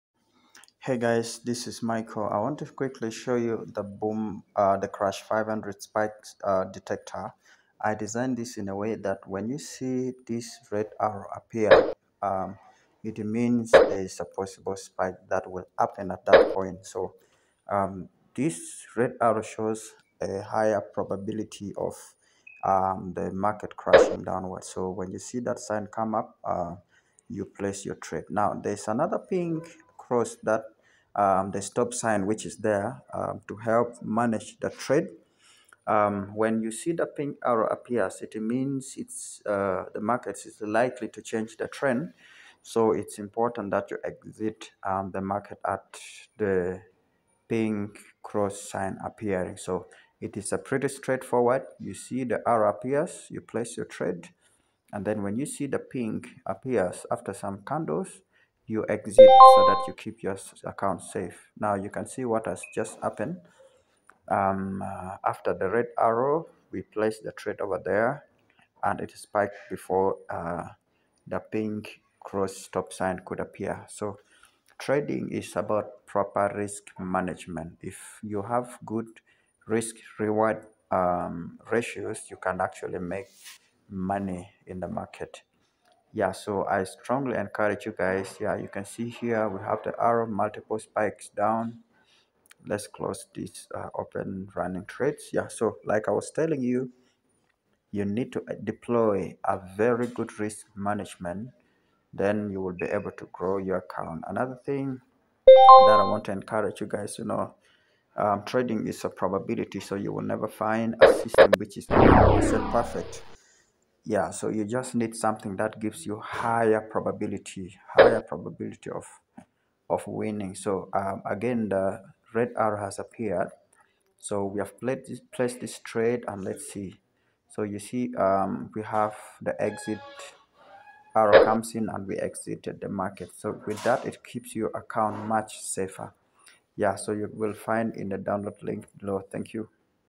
CRASH500 SPIKE DETECTOR
This spike detector is a pretty straightforward strategy based on the crash market trends. If you see the red arrow, place your trade... when the pink stop sign appears with the spike, exit.
Please, note that trading Markets are probability events largely meaning that no trading system is 100% perfect. Use proper risk management and you will succeed greatly.
Use m1 tf.